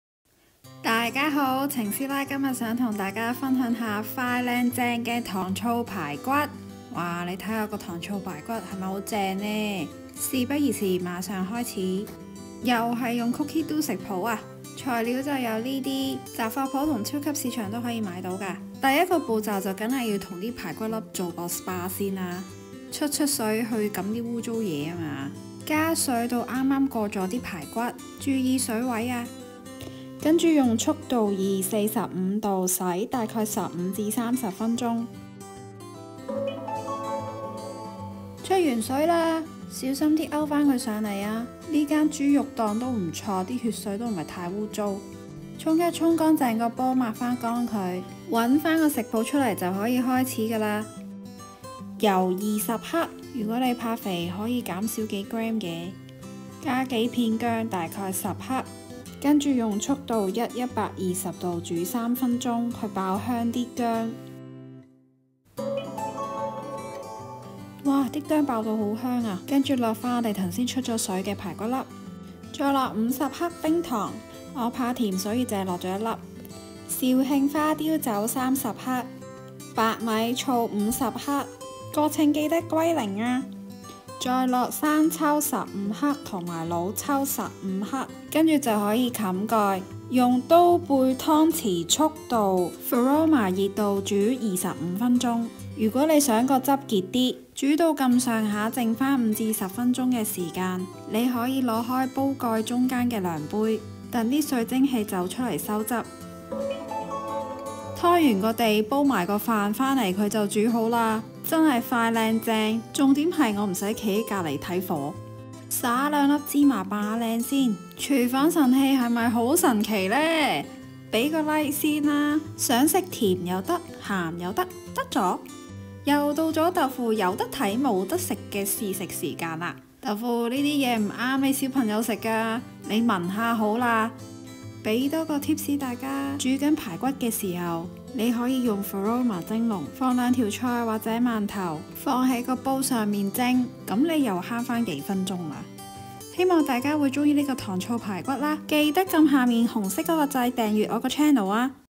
「糖醋排骨」(粵) | 太神奇了 | Easy Sweet & Sour Pork Ribs with Thermomix
想知道廚房神器訂購方式可以留言。